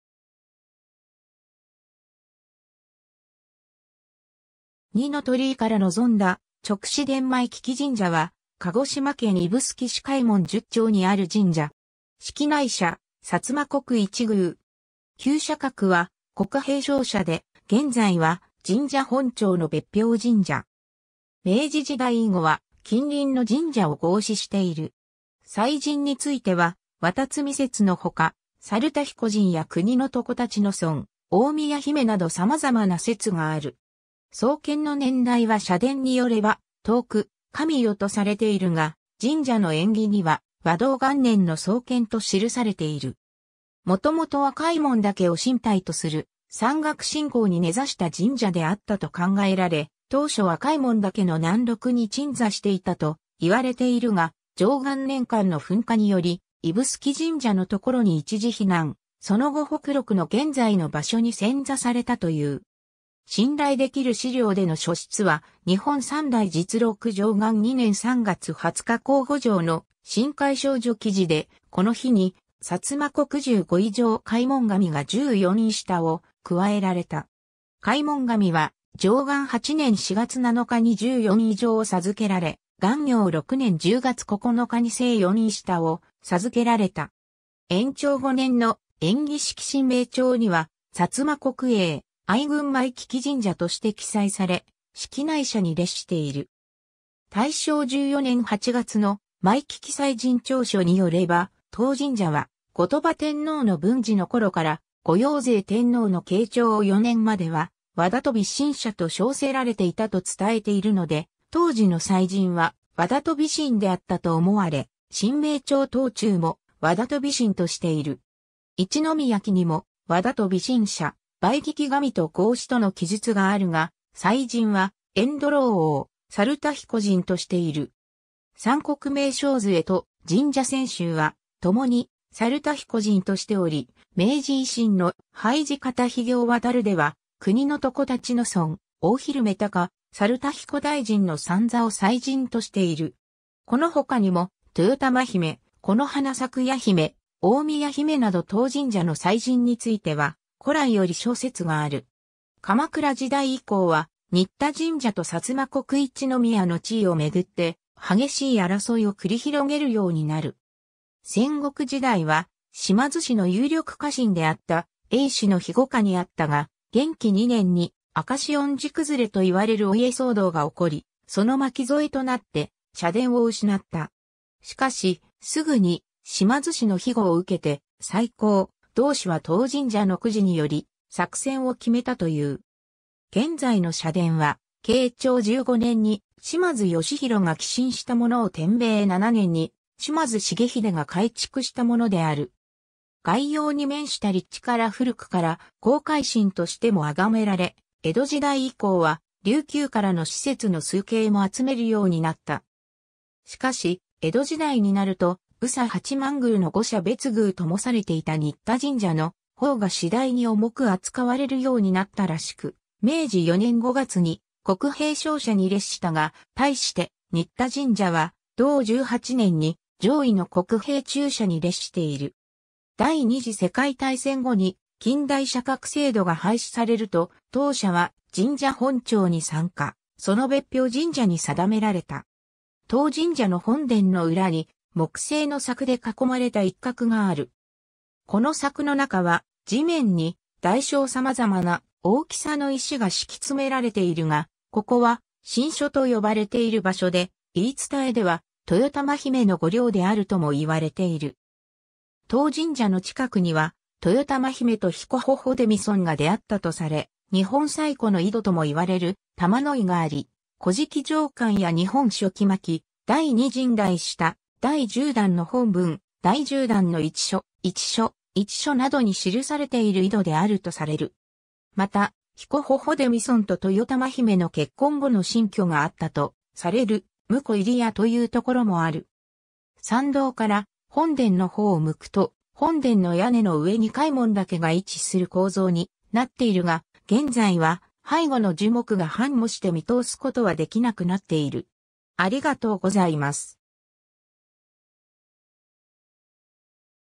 枚聞神社
二の鳥居から望んだ勅使殿 枚聞神社（ひらききじんじゃ）は、鹿児島県指宿市開聞十町にある神社。
式内社、薩摩国一宮。
旧社格は国幣小社で、現在は神社本庁の別表神社。
 明治時代以後は近隣の神社を合祀している。
祭神については海神（わたつみのかみ）説の他、猿田彦神や国常立尊、大宮姫など様々な説がある。
 創建の年代は社伝によれば、遠く神代とされているが、神社の縁起には，和銅元年(708年)の創建と記されている。
元々は開聞岳を神体とする山岳信仰に根ざした神社であったと考えられ、当初は開聞岳の南麓に鎮座していたといわれているが、貞観年間の噴火により揖宿神社の所に一時避難、その後北麓の現在の場所に遷座されたという。
 信頼できる史料での初出は『日本三代実録』貞観2年（860年）3月20日庚午条の神階昇叙記事で、この日に薩摩国従五位上開聞神が従四位下を加えられた。
開聞神は、貞観8年（866年）4月7日に従四位上を授けられ、元慶6年（882年）10月9日に正四位下を授けられた。
 延長5年（927年）の『延喜式神名帳』には「薩摩国穎娃郡 枚聞神社」として記載され、式内社に列している。
 大正14年8月の「枚聞祭神調書」によれば、当神社は後鳥羽天皇の文治の頃から後陽成天皇の慶長4年までは、和田都美神社と称せられていたと伝えているので、当時の祭神は和田都美神であったと思われ、「神名帳頭注」（1503年）も和田都美神としている。
「一宮記」にも「和田都美神社、枚聞神と号し」との記述があるが、祭神は塩土老翁猿田彦神としている。
「三国名勝図会」と「神社撰集」は共に猿田彦神としており、明治維新の「廃寺方被仰渡」では国常立尊、大日孁貴、猿田彦大神の三座を祭神としている。
この他にも豊玉姫、木花開耶媛、大宮姫など当神社の祭神については古来より諸説がある。
 鎌倉時代以降は新田神社（薩摩川内市）と薩摩国一宮の地位を巡って激しい争いを繰り広げるようになる。
戦国時代は島津氏の有力家臣であった頴娃氏の庇護下にあったが、元亀2年（1571年）に「証恩寺崩れ」と言われるお家騒動が起こり、その巻き添えとなって社殿を失った。
しかし、すぐに島津氏の庇護を受けて再興、同氏は当神社の籤により作戦を決めたという。
現在の社殿は慶長15年（1610年）に島津義弘が寄進したものを天明7年（1787年）に島津重豪が改築した物である。
 外洋に面した立地から古くから「航海神」としても崇められ、江戸時代以降は琉球からの使節の崇敬も集めるようになった。
 しかし江戸時代になると、宇佐八幡宮の五社別宮ともされていた新田神社の方が次第に重く扱われるようになったらしく、明治4年（1871年）5月に国幣小社に列したが、対して新田神社は同18年（1885年）に上位の国幣中社に列している。
 第二次世界大戦後に近代社格制度が廃止されると、当社は神社本庁に参加、その別表神社に定められた。
 当神社の本殿の裏に、木製の柵で囲まれた一角がある。
この柵の中は、地面に大小様々な大きさの石が敷き詰められているが、ここは「清所」と呼ばれている場所で、言い伝えでは豊玉姫の御陵であるともいわれている。
 当神社の近くには、豊玉姫と彦火々出見尊が出会ったとされ、日本最古の井戸ともいわれる「玉の井」があり、古事記 上巻や日本書紀巻第二 神代下・第十段の本文、第十段の一書（第一）、一書（第二）、一書（第四）などに記されている井戸であるとされる。
また彦火々出見尊と豊玉姫の結婚後の新居があったとされる「婿入谷」という所もある。
 参道から本殿の方を向くと、本殿の屋根の上に開聞岳が位置する構造になっているが、現在は背後の樹木が繁茂して見通すことはできなくなっている。